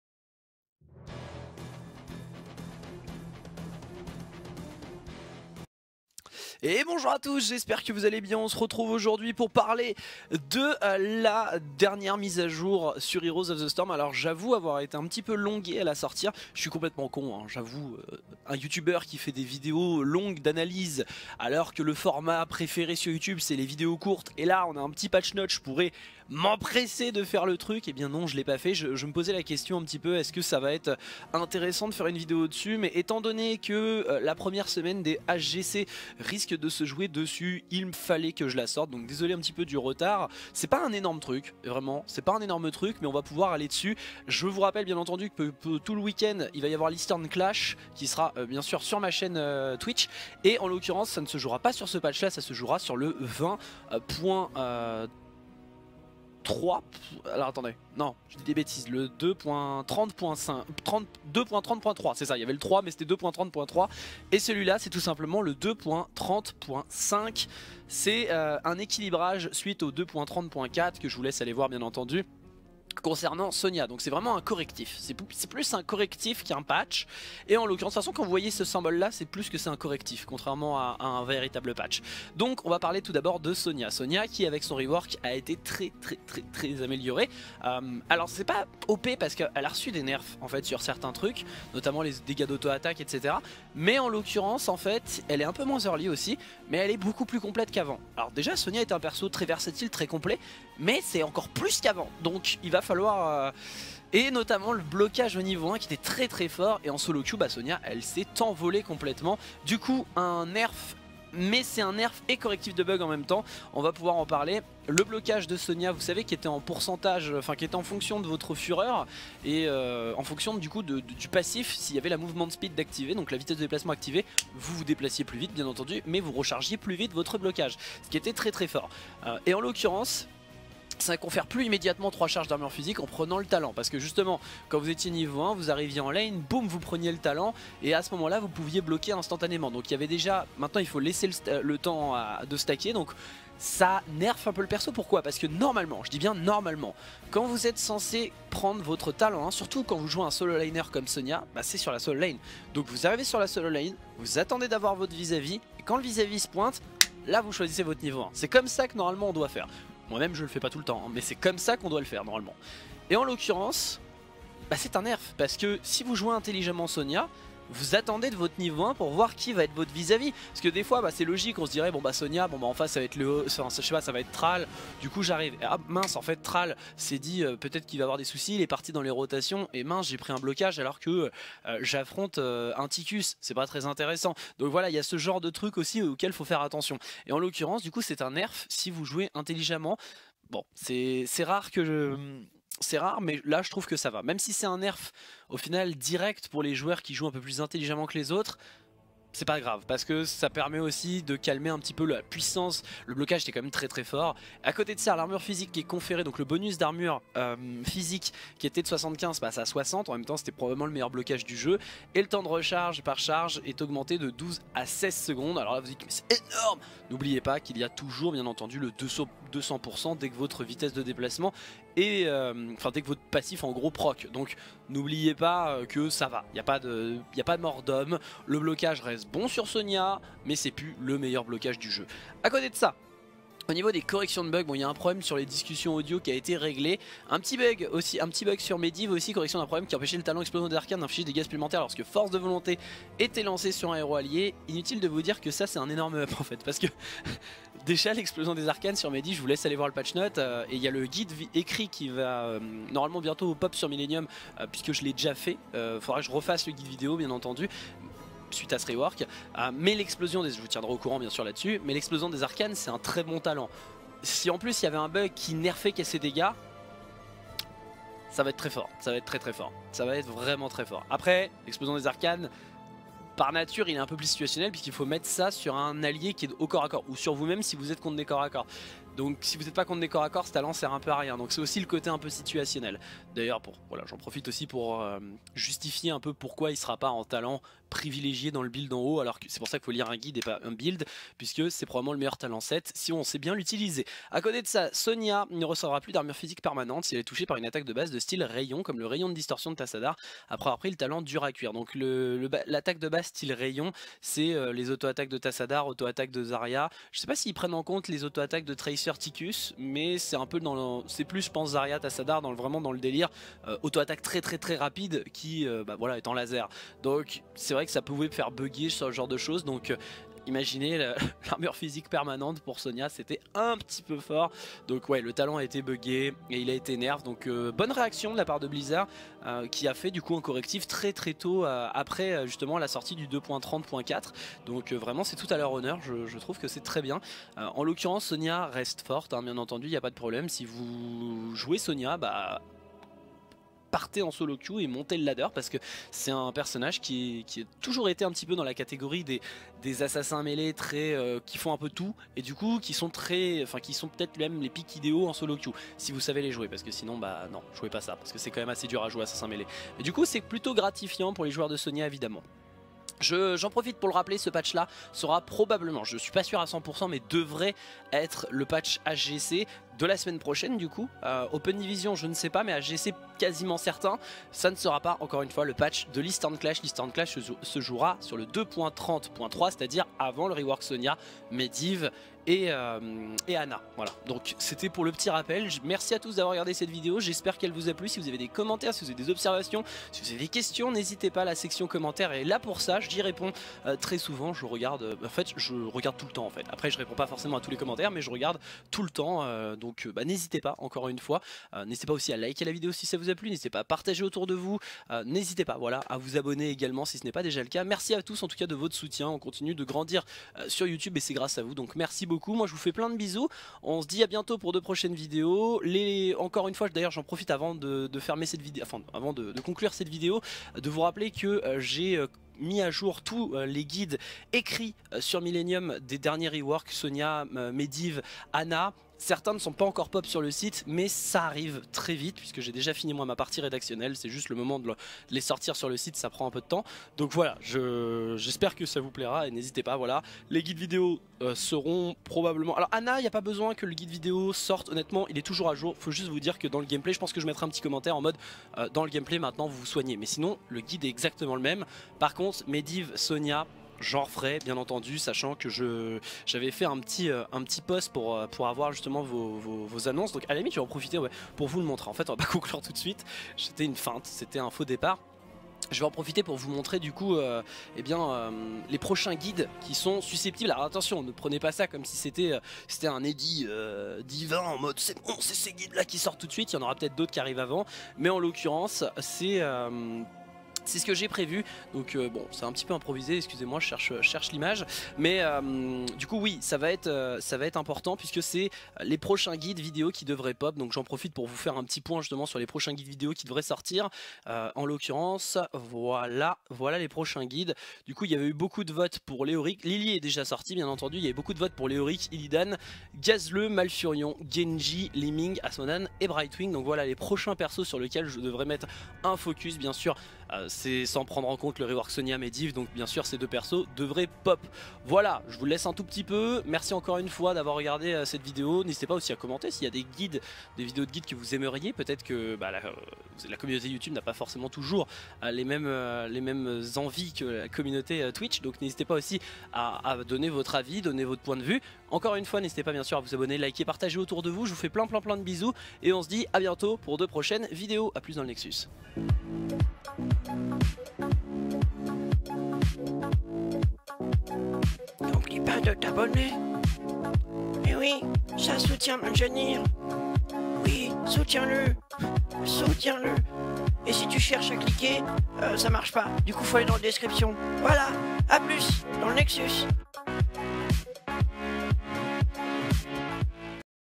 [HotS] Patch HGC Correctif Sonya 2.30.5
Voici l'analyse du patch 2.30.5 (patch du 10 Mars 2018)
Qui vraisemblablement sera le patch de la reprise des HGC (Partie 2 de la Phase 1 2018)

0:30 Rappel patchs HGC
1:23 Sonya
3:53 Bugs
6:55 Guides en cours et qui arrivent